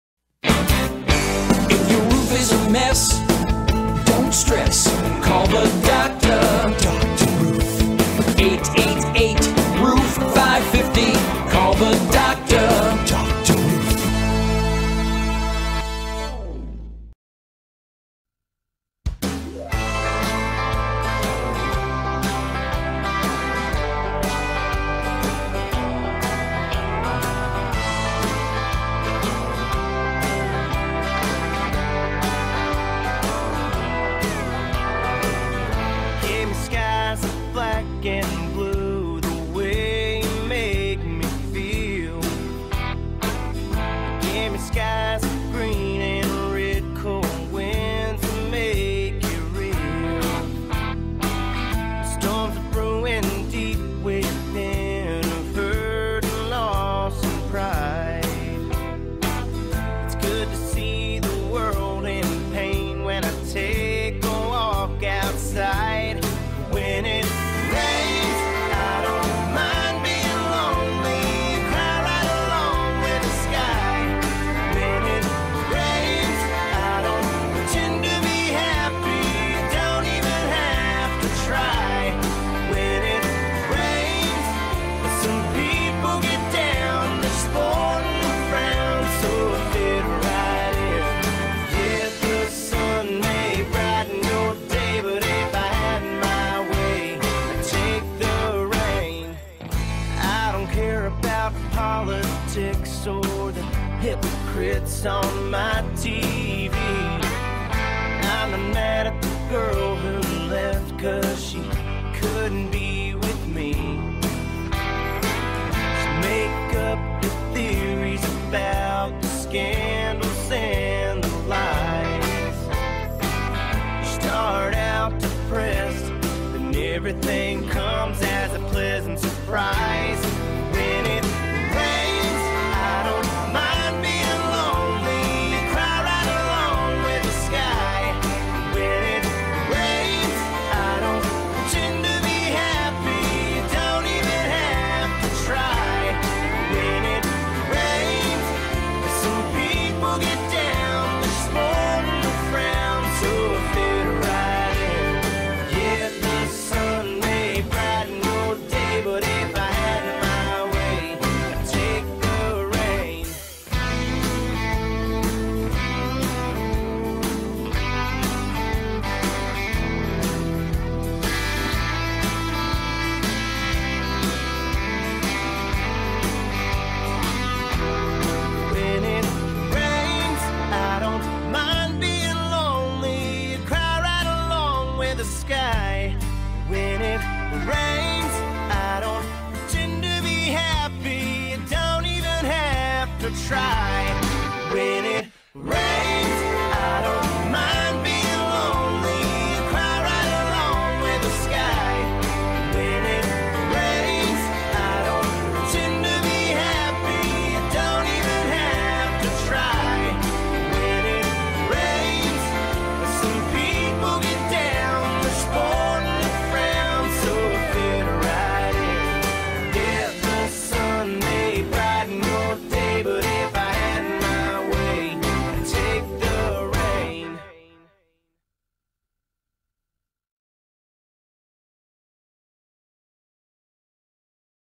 Devine Project
Strip and re-roof a lovley home in Lakeville, MA
Music in this video is by the Eli Young Band... great song guys!